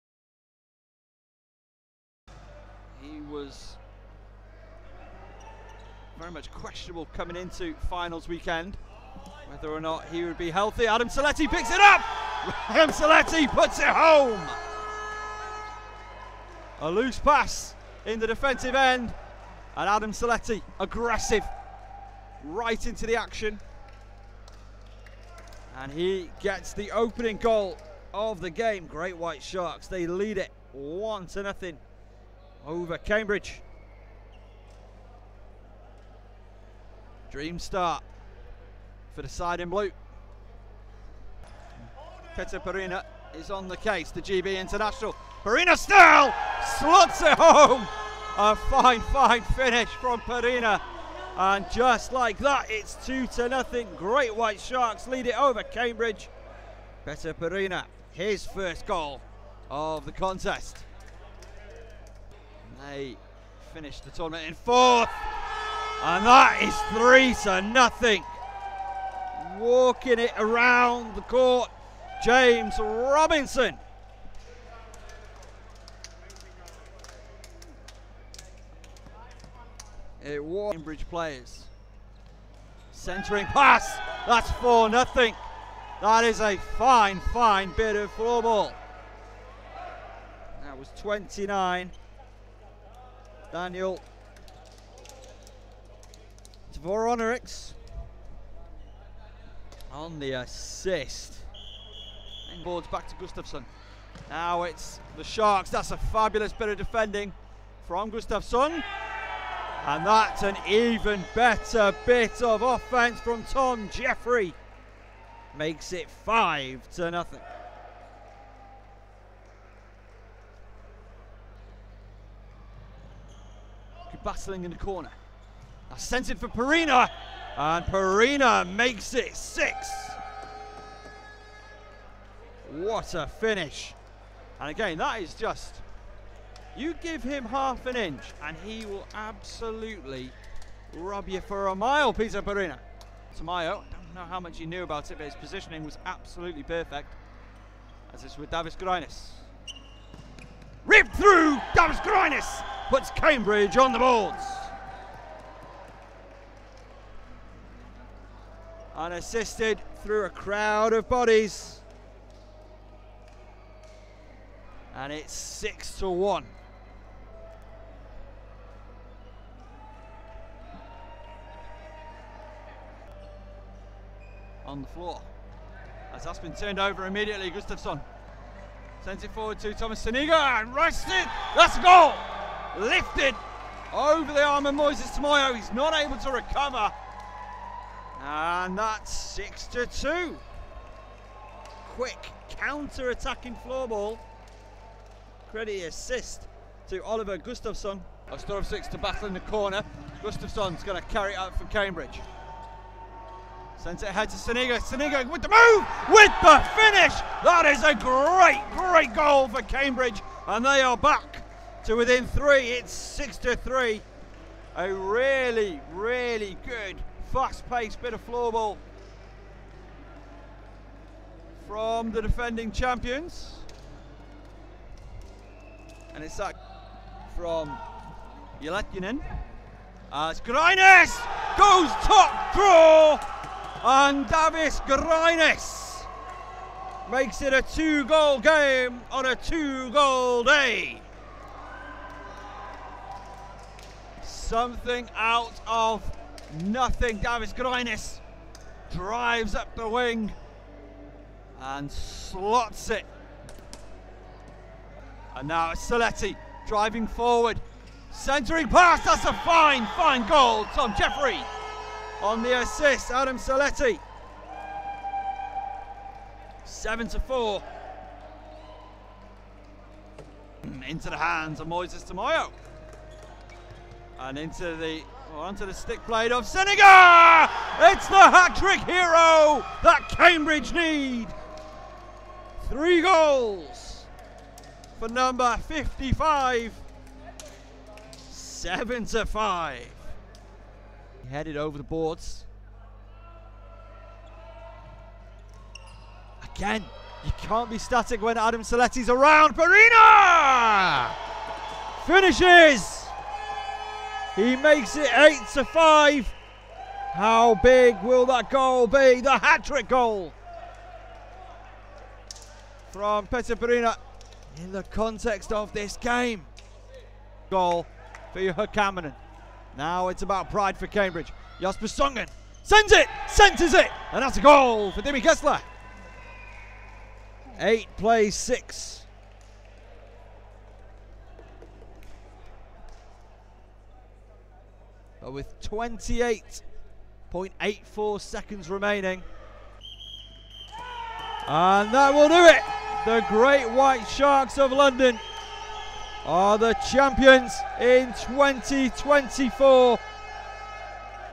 GWS vs Cambridge | FINAL Highlights | UKFF National Finals 2024 | Floorball
Relive all the goals as third seed Cambridge took on the undefeated London Great White Sharks to decide the 2024 UKFF National Final.

Photo Credit: Graham Hodges (GrahamHodges3)